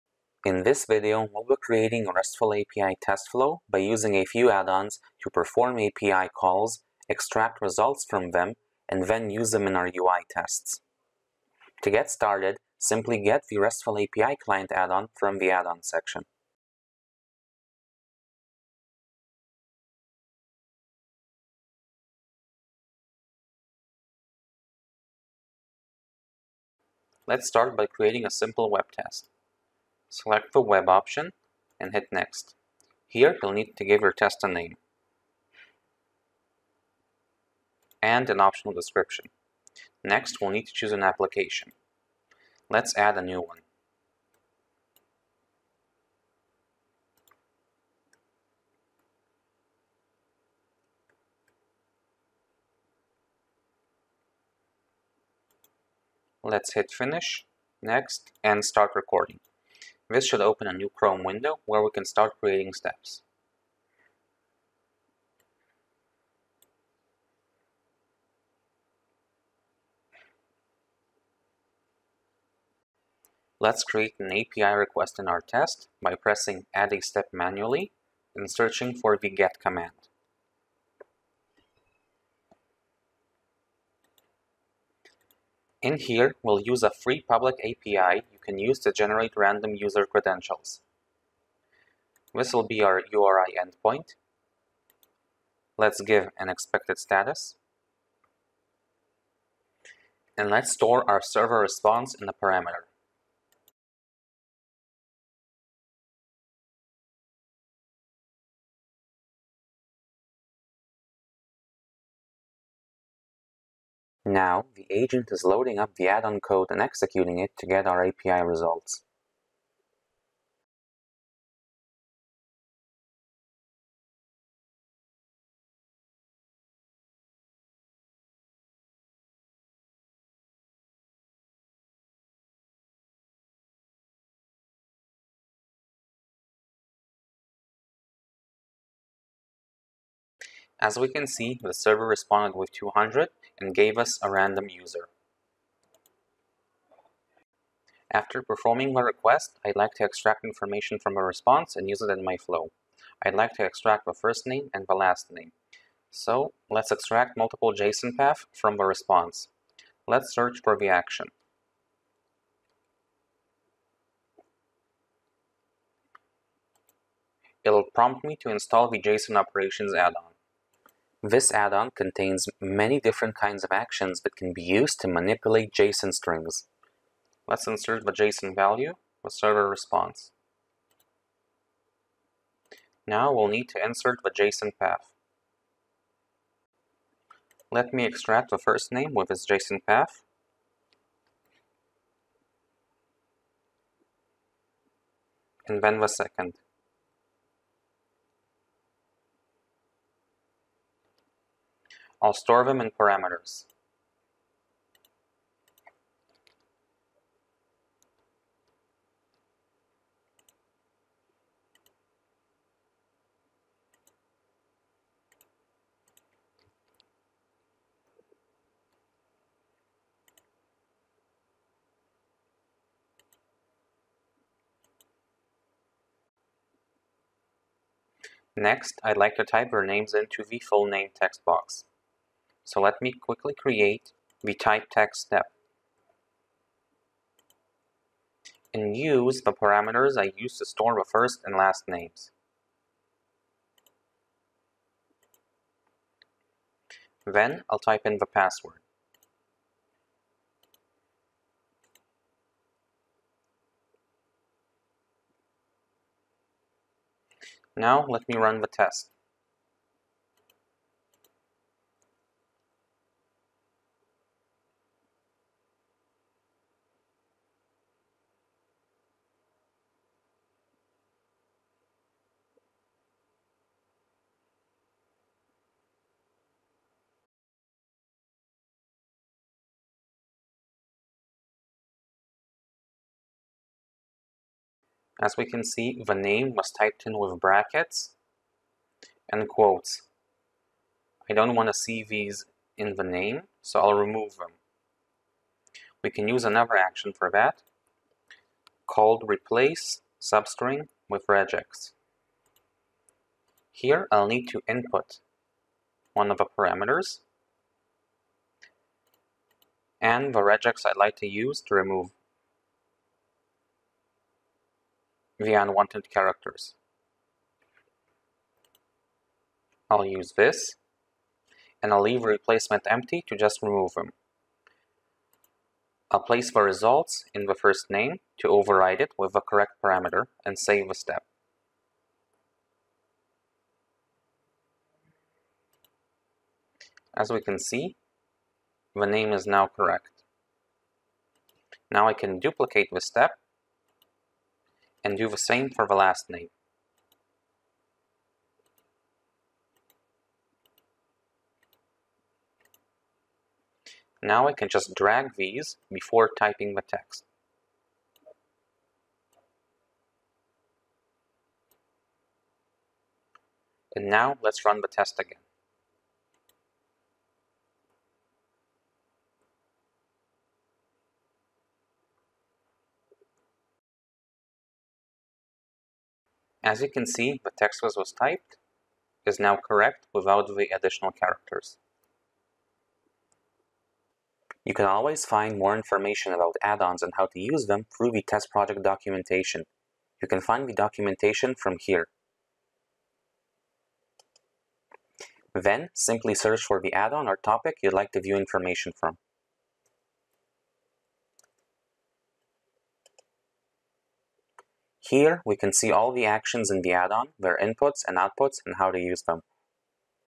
End to End API Testing Flow with TestProject
In this video, we will create an end-to-end RESTful API test flow: perform API calls, extract results from them, and use them in our UI test. We will see how to use TestProject's RESTful API Addon In order to extract the response, we will use JSON operations Addon In order to "clean" the name, we will use the String Utils Addon

TestProject Team